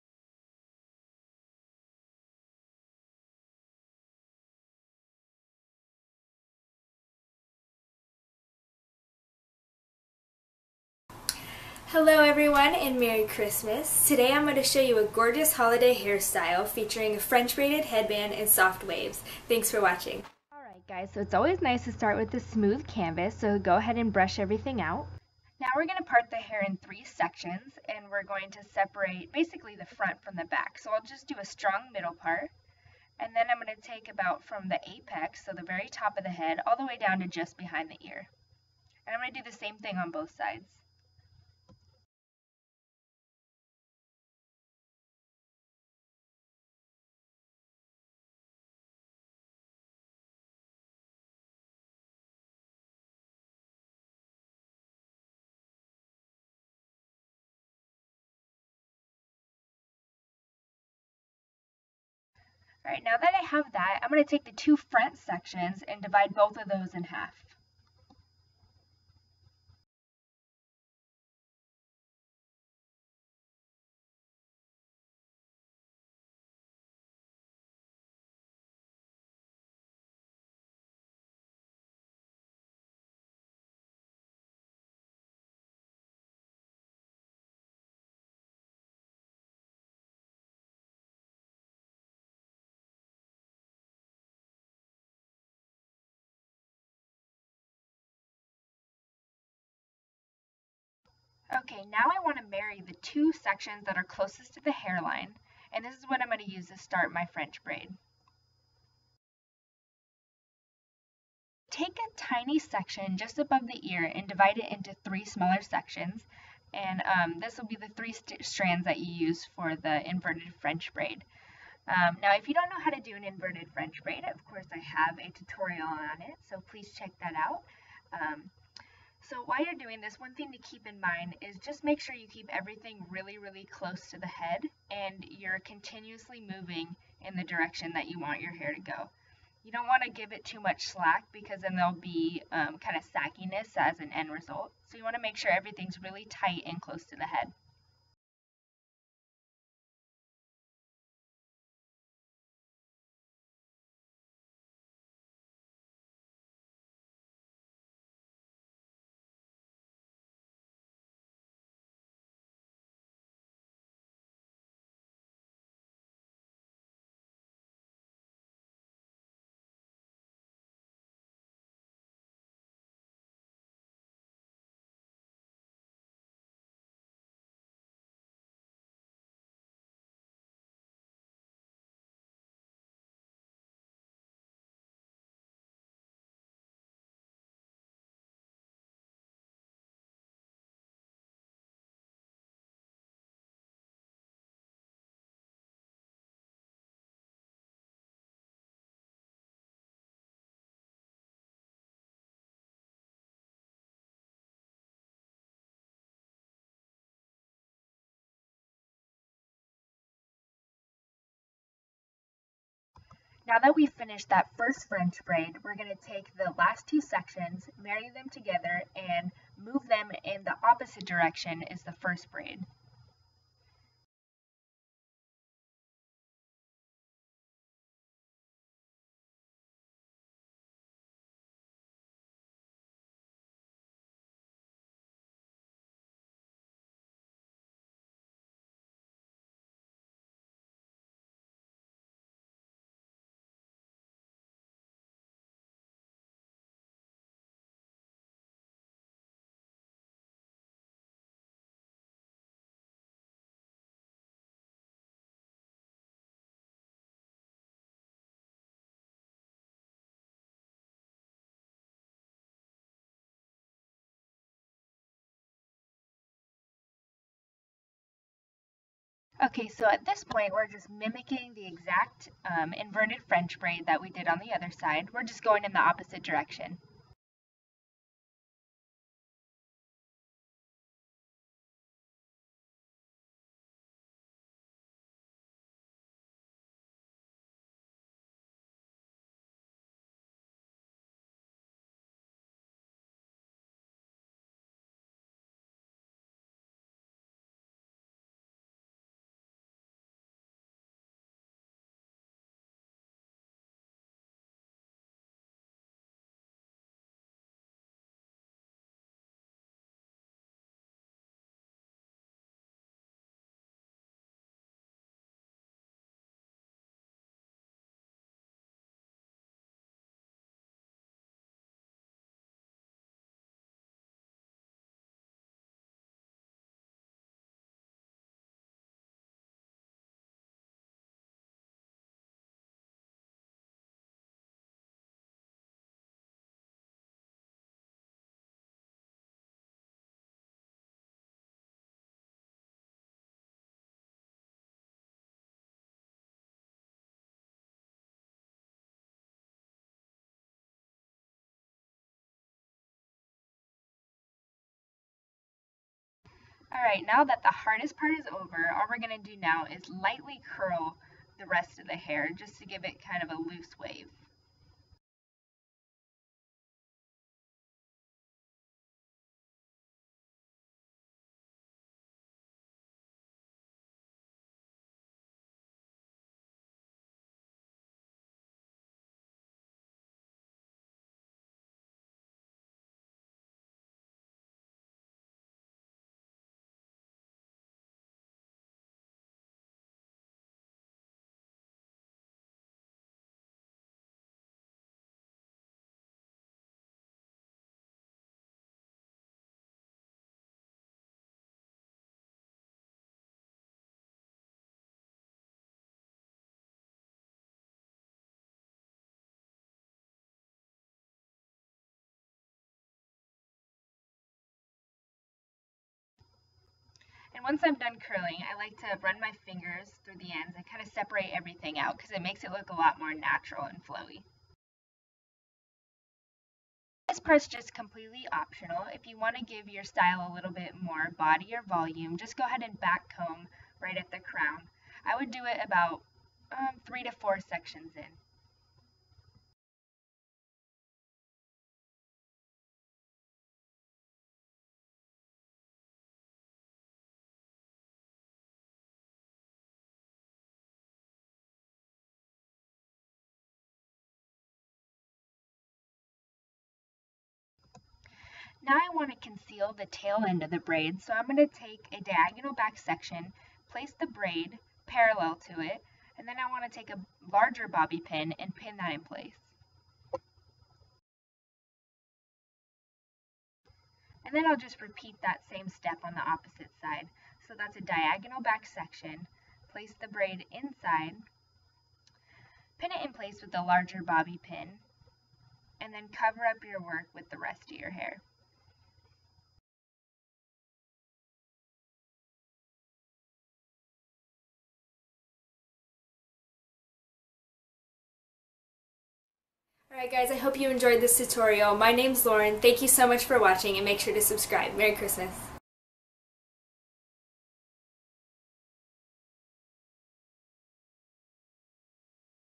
Braided headband holiday hairstyle!
A step by step tutorial showing how I create an inside out french braided headband! Great for New Years Eve!:) If you like this one, check out this variation!. or this! h